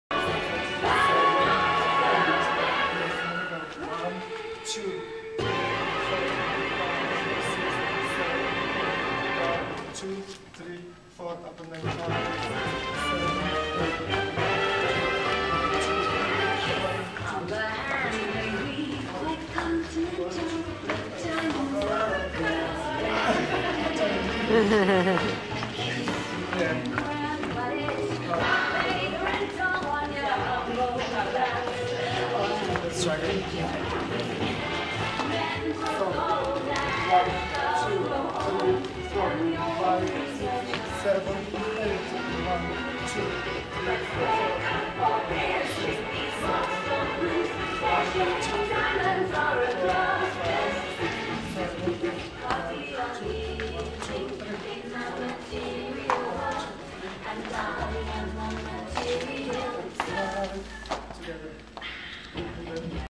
Kristin Hedlund and Nikolay Kralev Rehearsal
Kristin and Nikolay show some fancy footwork in rehearsal for the Feb. 25 Dancing with the Stars Arizona event.